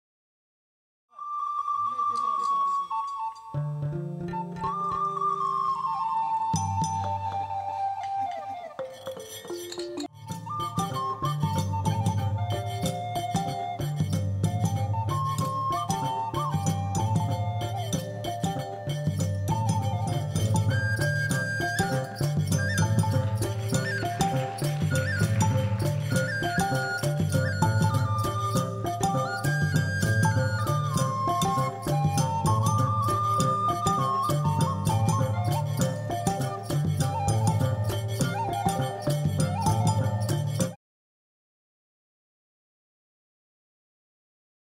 Yamoussa Juggling
Yamoussa Bangoura from Kalabante performs with clubs.